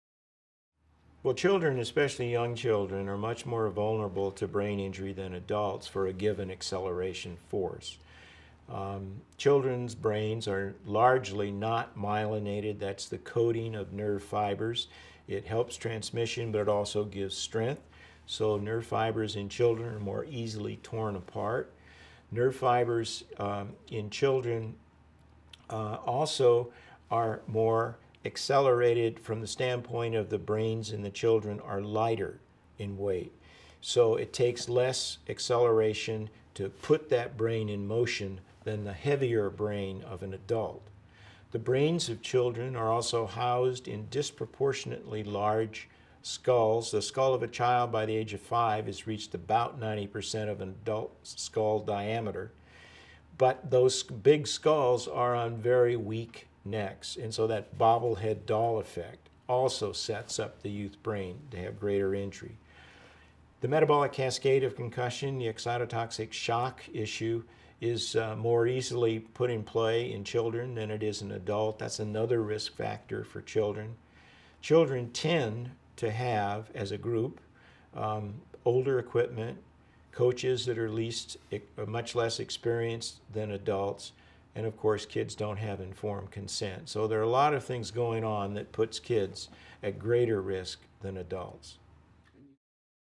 Why Are Kids at Greater Risk of Sports Concussion than Adults?
Dr. Robert Cantu explains why the brains of children are more vulnerable to concussion — from nerve fibers in the brain that are more easily torn apart to the size discrepancy between their large skulls and little necks.